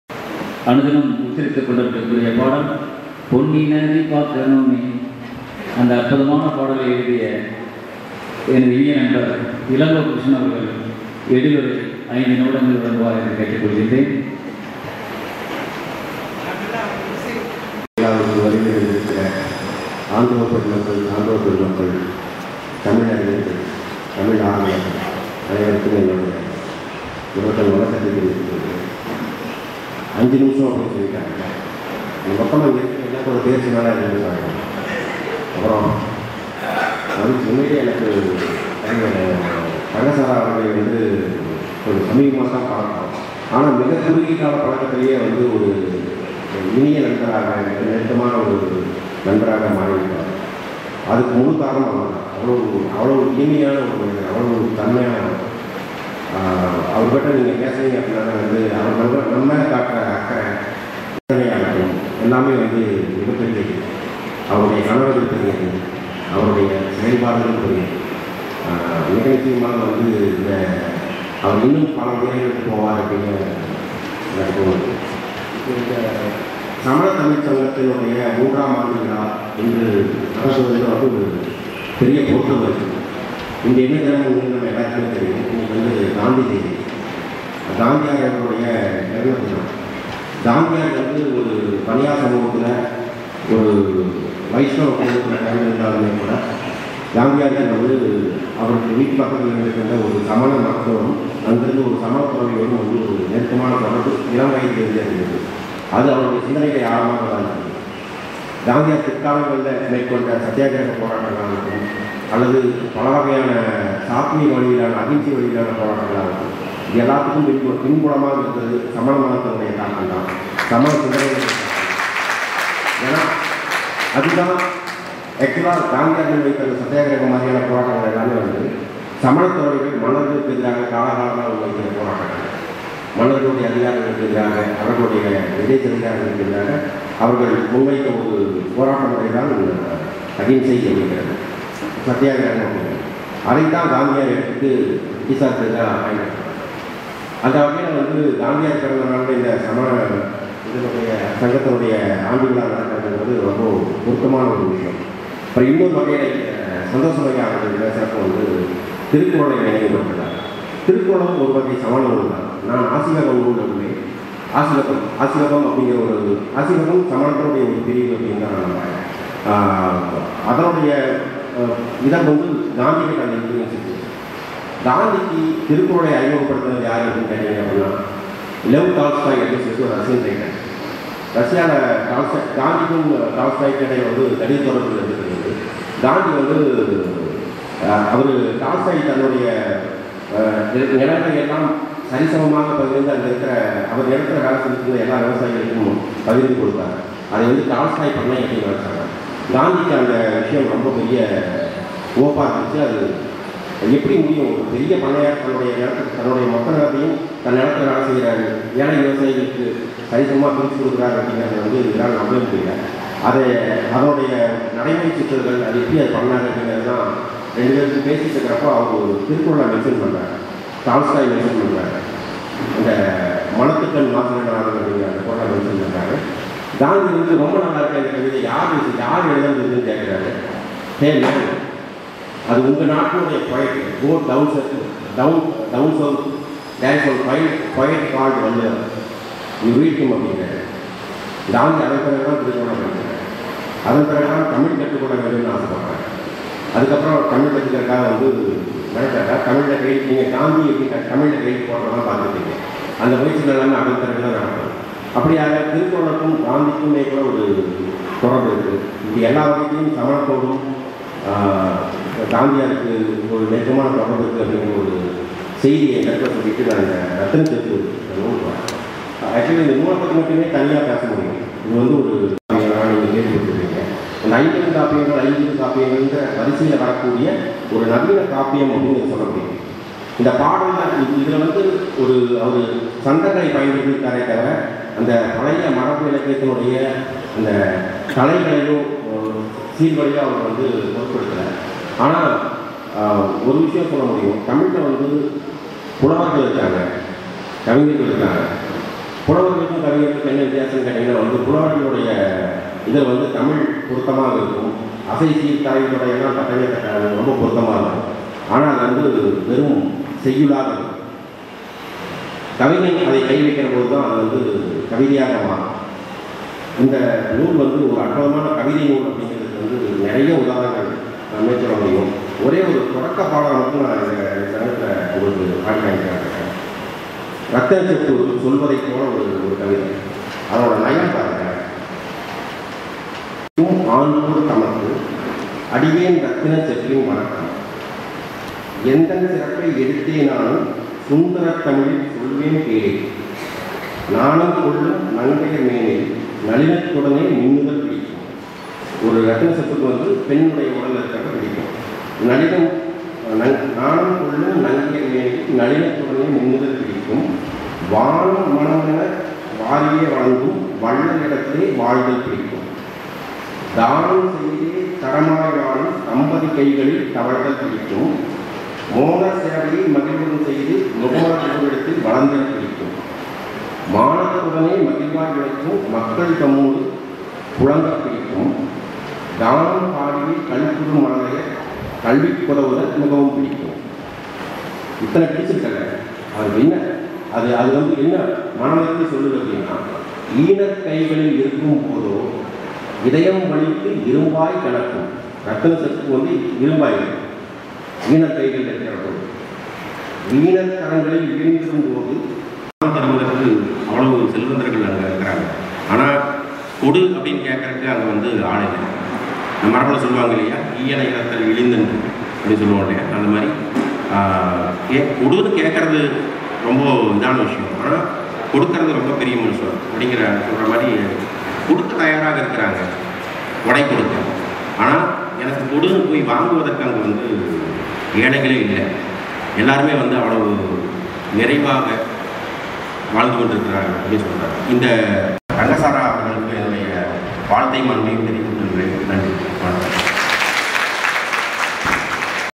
21 சிறப்புரை கவிஞர் இளங்கோ கிருஷ்ணன்
சமணத் தமிழ்ச் சங்கம் - 3ம் ஆண்டு விழா
02 அக்டொபெர் 2023